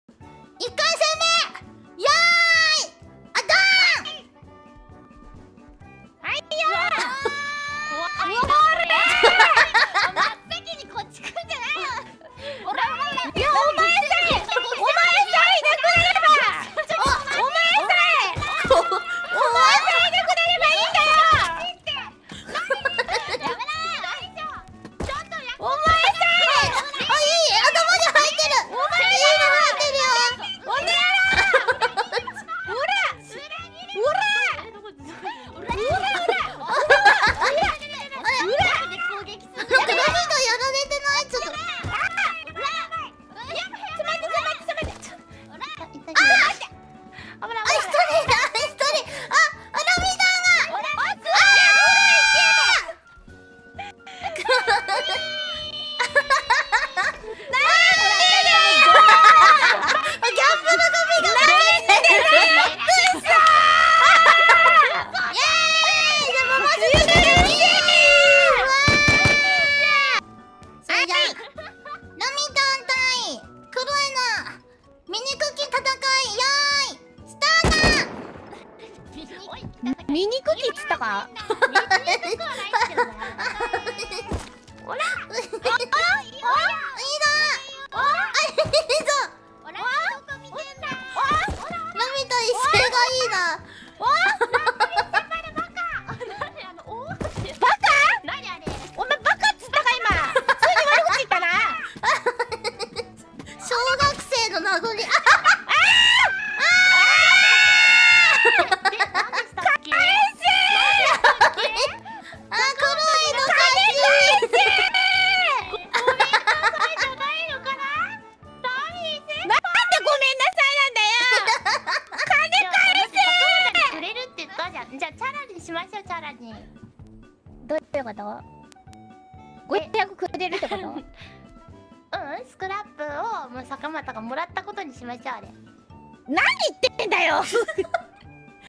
2022/09/15 みこ鬪技場Fight1
みこ鬪技場Fight1
1-1(ねねち vs ギャンブル組)
やっばりギャンブルしないほうがいいかもね😂
1-2(クロヱ vs ラミィ)
ギャンブル廃人組😎
youtu.be/4Gy6Idt6Dz0